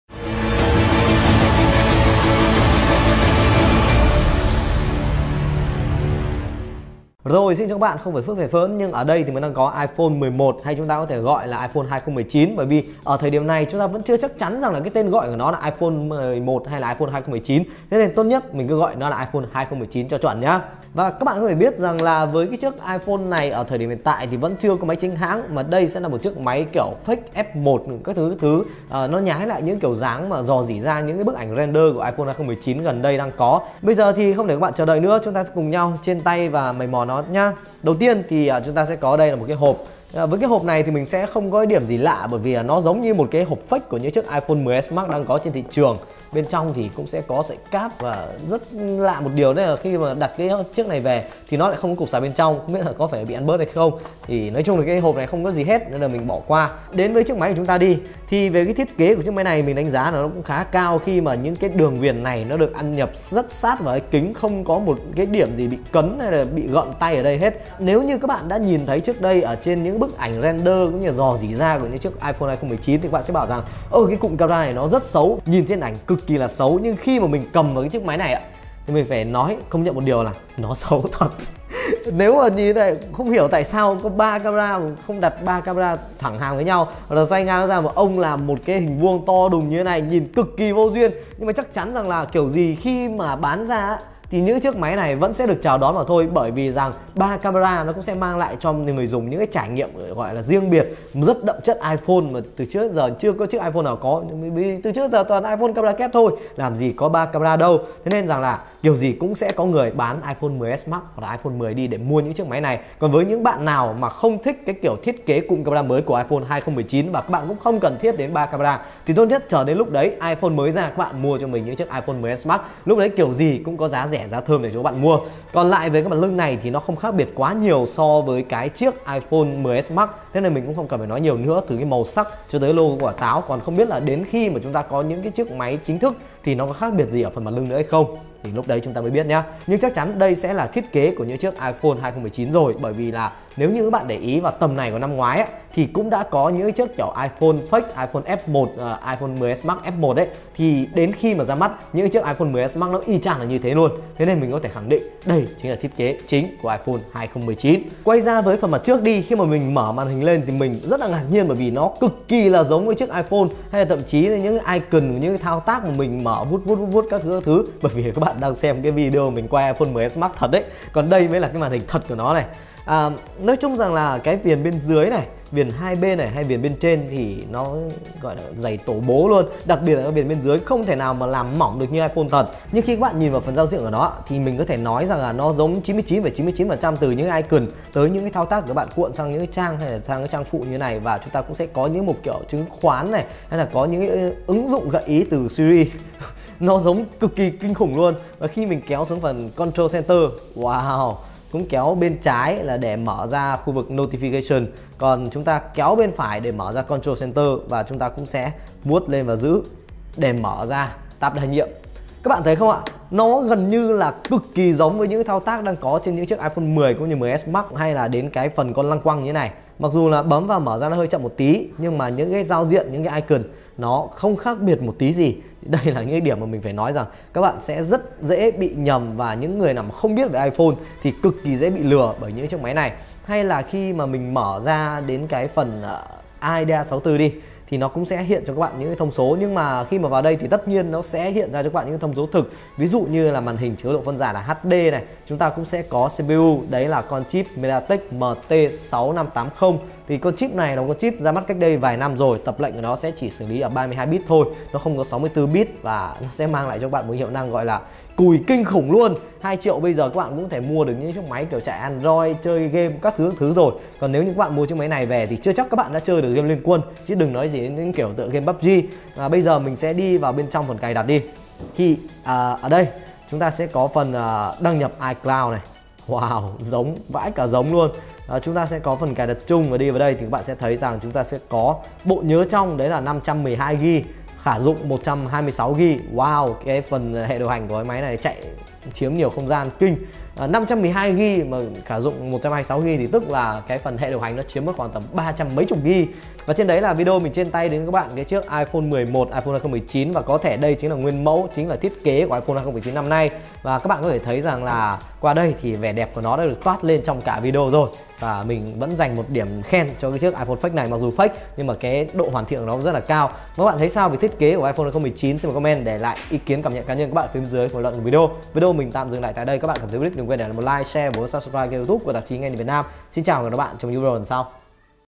iPhone XI, iPhone 11 512GB giá 2 triệu là đây chứ đâu
iPhone XI hay iPhone 2019 hiện đang đón nhận được rất nhiều sự quan tâm của người dùng cũng như giới truyền thông công nghệ và hiện tại đã có những nguyên mẫu F1 được bán ra, hãy cùng nghenhinvietnam trên tay sản phẩm này!